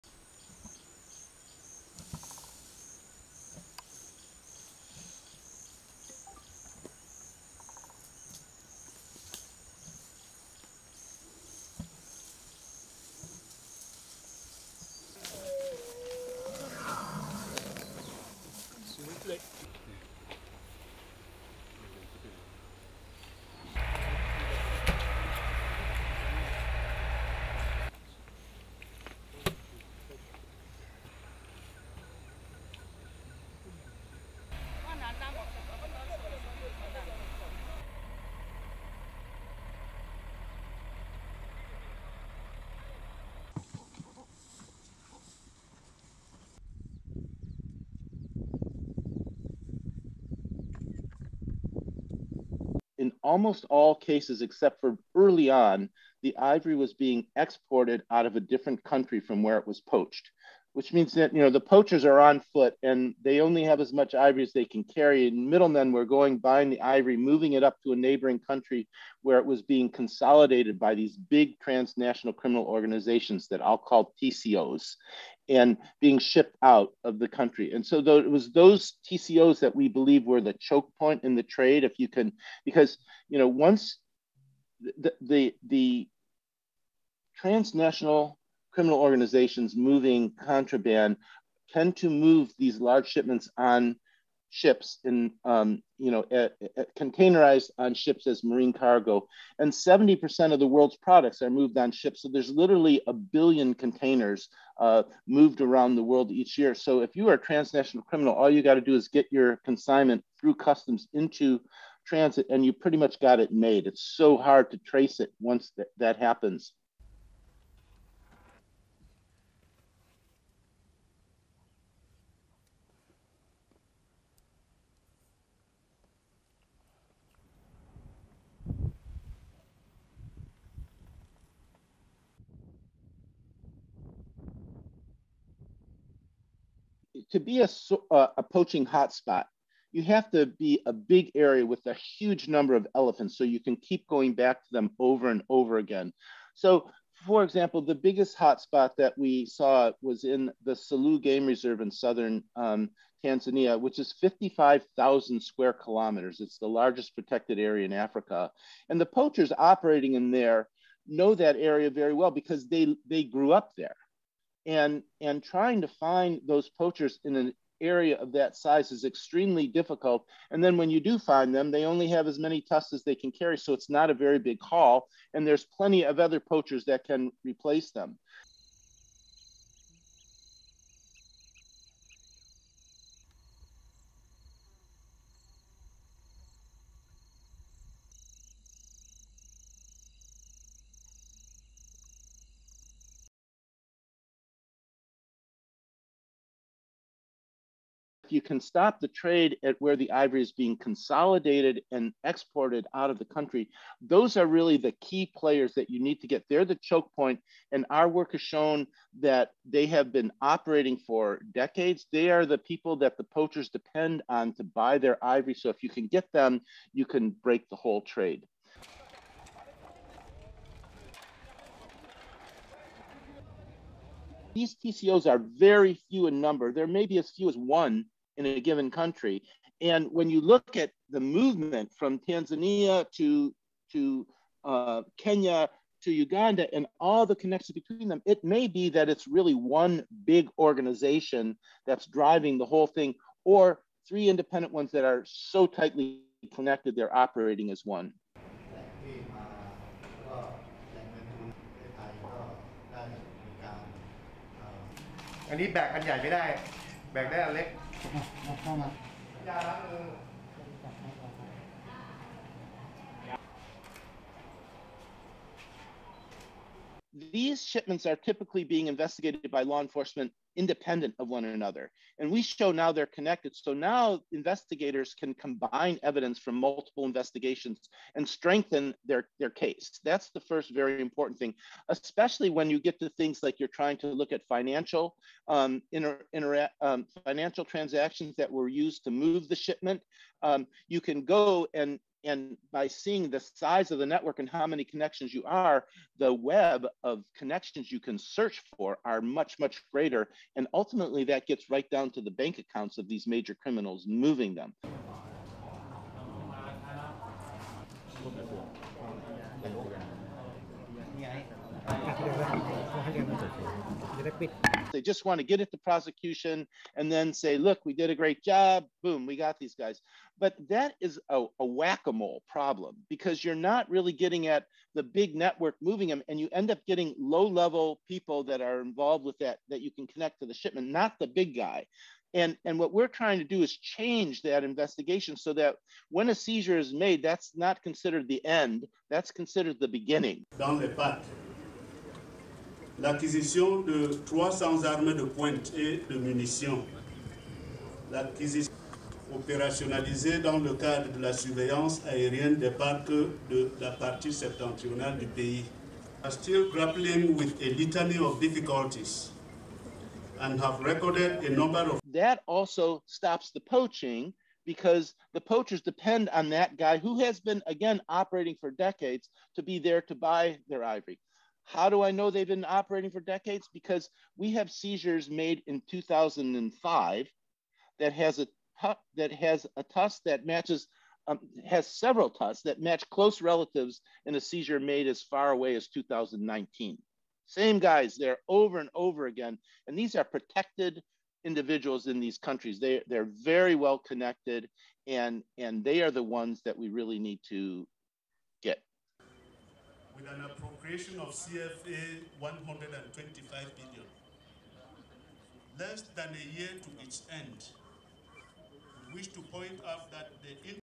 Scientists use ivory tusk DNA data to map poaching networks
(14 Feb 2022) LEAD IN:
As few as three major criminal groups are responsible for smuggling the vast majority of elephant ivory tusks out of Africa.
A new study combines elephant DNA data and forensic evidence to map the surprising interconectedness of trafficking operations across the continent.

STORY-LINE:
In the Ivory Coast in west Africa a group of conservationists and veterinarians are moving this increasingly endangered forest elephant to an area where the animal is less likely to be targetted by poachers.
Scientists have been using DNA testing to track how ivory traffickers are operating.
As few as three major criminal groups are responsible for smuggling the vast majority of elephant ivory tusks out of Africa, according to a new study.
They've develop a combined genetic and statistical method to determine the origin of poached ivory and they are collaborating with the Interpol Working Group on Wildlife Crime to investigate the origins of all major ivory seizures in the recent past.
Researchers used analysis of DNA from seized elephant tusks and forensic evidence such as phone records, license plates, financial records and shipping documents to map trafficking operations across the continent and better understand who was behind the crimes.
The study was published (Monday 14 February)  in the journal Nature Human Behavior.
They've identified key locations where ivory is poached, packed in shipping containers, then moved by truck or rail to port cities – and how traffickers have shifted operations over time in response to law enforcement.
Current trafficking hubs exist in Kampala, Uganda; Mombasa, Kenya; and Lome, Togo.
Co-author Professor Samuel Wasser from the University of Washington says the focus has been on large shipments of over half a metric ton.
They have been tracing what they describe as "transnational criminal organizatons" or TCOs.
Wasser says the contraband is moved in large amounts in shipping containers to ports outside of the countries where the poaching happens.
He says: "In almost all cases, except for early on, the ivory was being exported out of a different country from where it was poached, which means that, you know, the poachers are on foot and they only have as much as they can carry, and middlemen were going buying the ivory, moving it up to a neighbouring country where it was being consolidated by these big transnational criminal organisations, that l'll call TCOs, and being shipped out of the country. And so it was those TCOs that we believe were the choke point in the trade, if you can, because once the transnational criminal organisations, moving contraband tend to move these large shipments on ships in containerised on ships as marine cargo. And 70 percent of the world's products are moved on ships, so there's literally a billion containers moved around the world each year. So if you are a transnational criminal, all you got to do is get your consignment through customs into transit and you pretty much got it, made it so hard to trace it once that happens."
Wasser says the poaching is concentrated in a few carefully chosen areas.
Each year, some 500 metric tons (1.1 million pounds) of poached elephant tusks are shipped from Africa, mostly to Asia.
Wasser hopes the findings will help law enforcement officials target the leaders of these networks instead of low-level poachers who are easily replaced by criminal organizations.
According to Wasser all of the vast illegal trade in ivory is being organized by a very few well protected criminals.

Traffickers that smuggle ivory also often move other contraband, the researchers found.